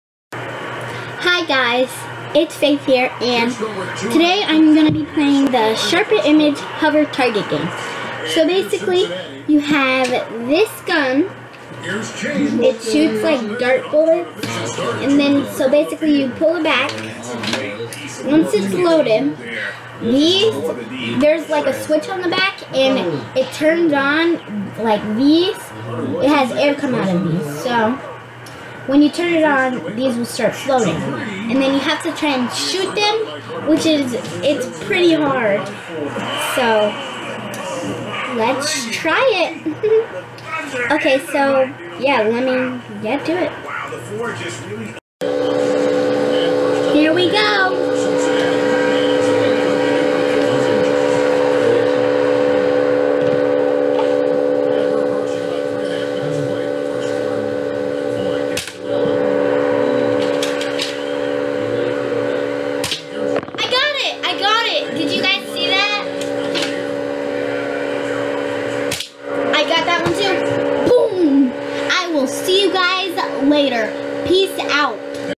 Trying To Shoot Balls That Are Floating!!
This Is The Sharper Image Hover Target Game!!